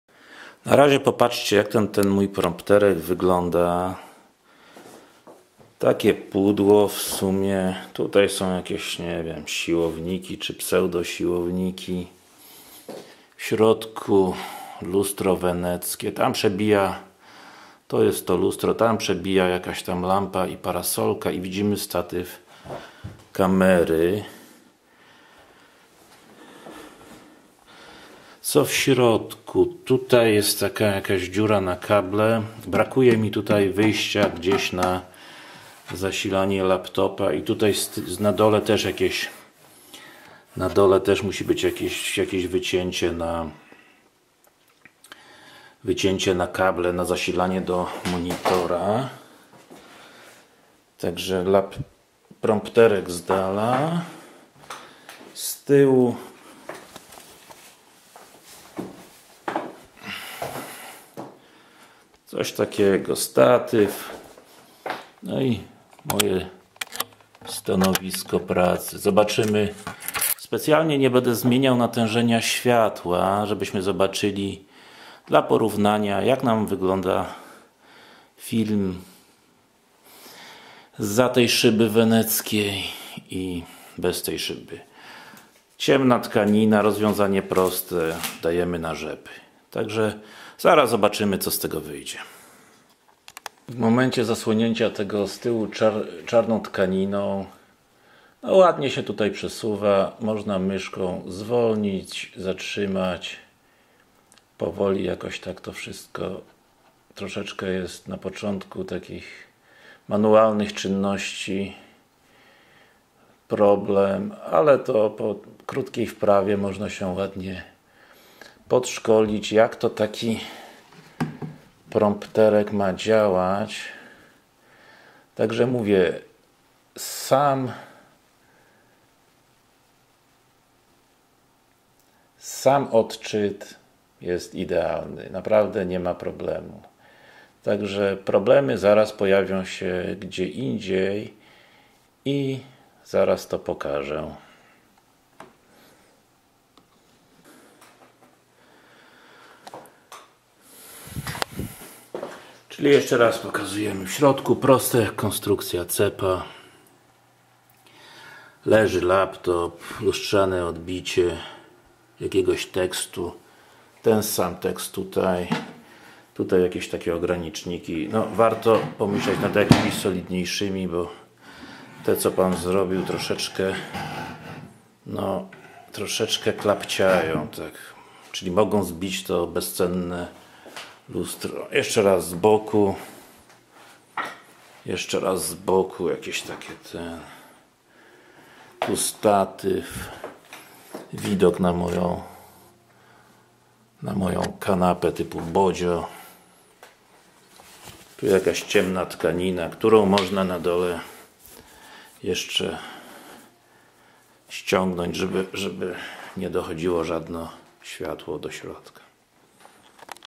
Prompter : Konstrukcja Domowa wymagająca ulepszeń !!
Mój prompter domowej konstrukcji, typu zrób to sam, mający w zasadzie jedną wadę. Lustro weneckie jest zbyt słabo przepuszczalne dla światła, co powoduje, że obraz jest bardzo ciemny.

Na razie jestem na etapie szukania lepszego lustra, a to wideo jest dla osób, które chcą uniknąć początkowych kłopotów i dość wysokiej ceny za niewłaściwe lustro. Jeszcze na dobrą sprawę nie wiem jakie parametry ma spełniać to właściwe, ale jeżeli je zdobędę, to zrobię jakiś film porównawczy tych teleprompterów.